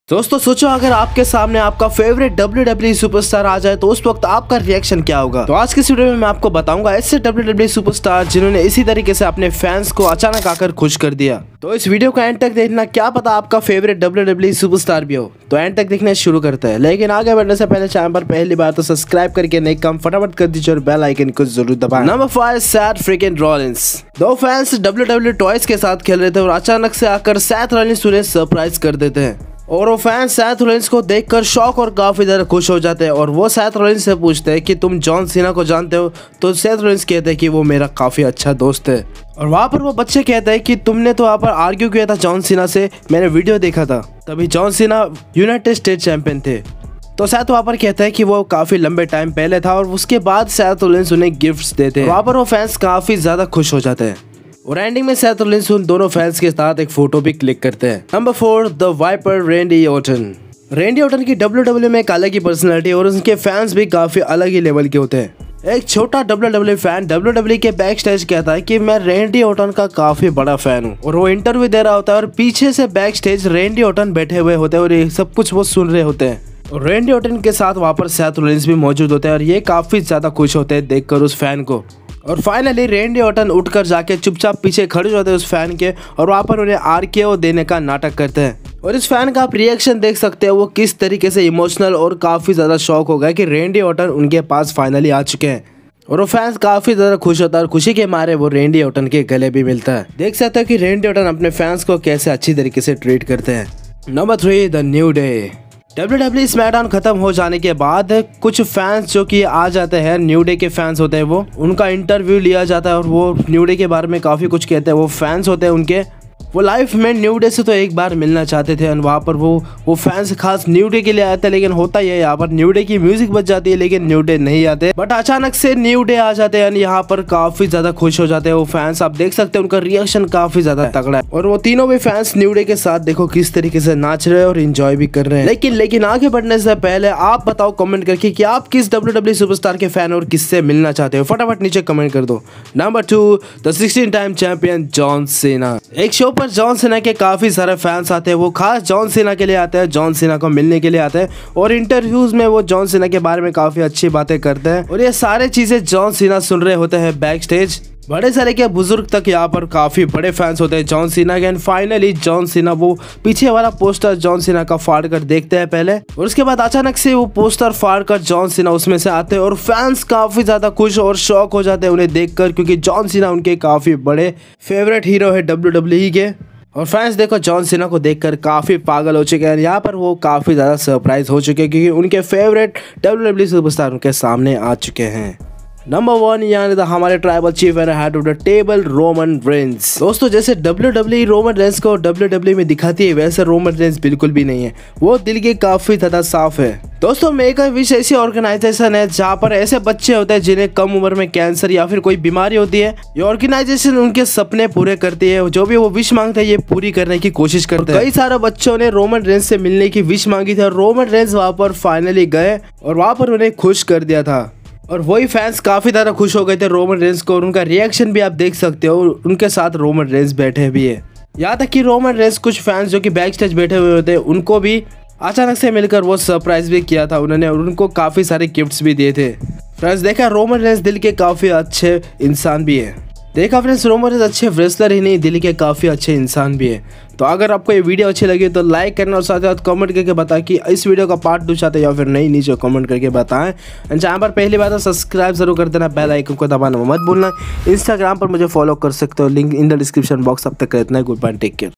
When Famous WWE Superstars Surprising Their Fans | Roman Reigns, John Cena,Randy Ortan, Seth Rollins
Trending Now ►When Famous WWE Superstars Surprising Their Fans | Roman Reigns, John Cena,Randy Ortan, Seth Rollins.WWE Superstars Connected with fans with social media handles or virual meetup or wwe events

About ►"Wrestling Masala" This channel gives you Wrestling News, Rumors & Exclusive Stuff.